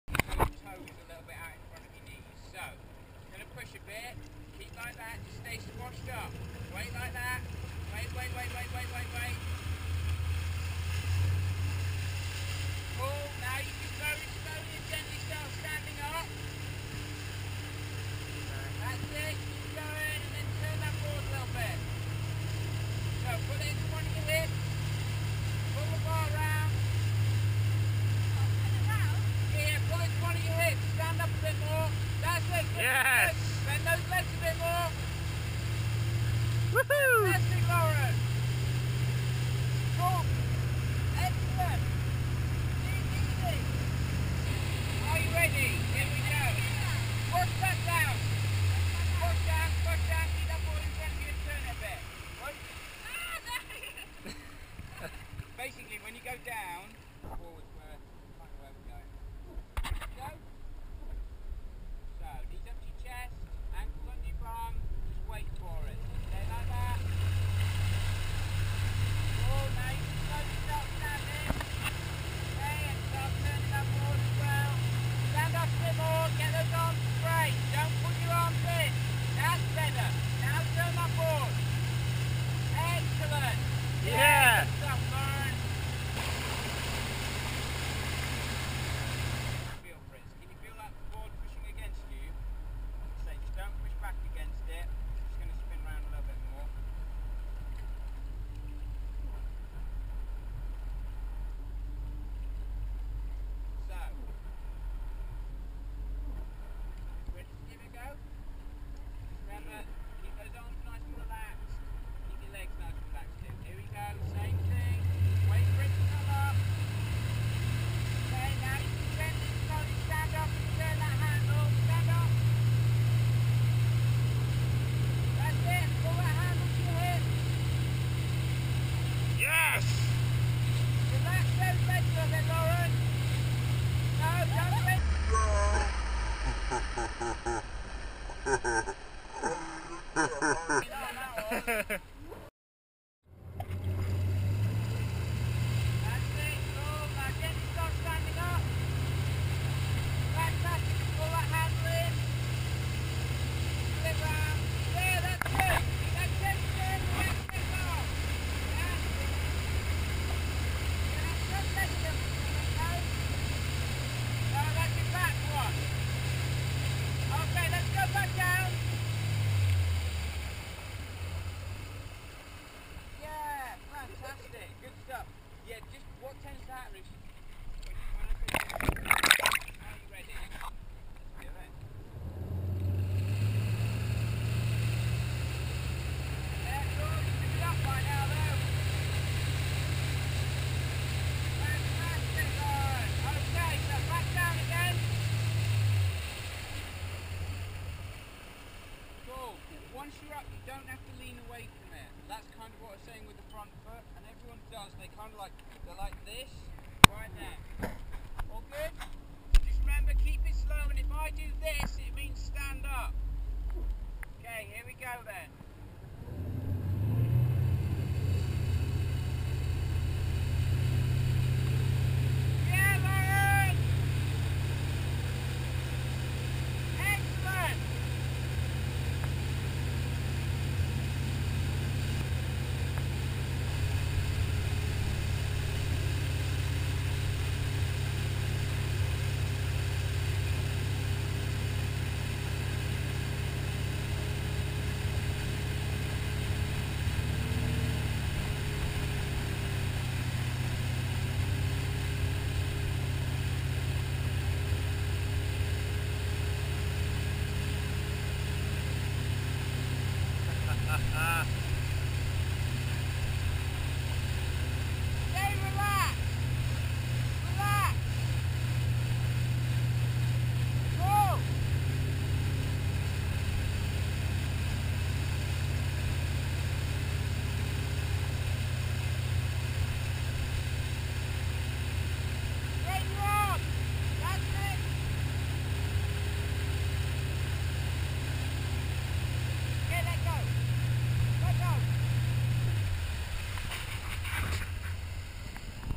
Lauren's Lesson
Lauren wakeboarding for the first time and making it seem easy.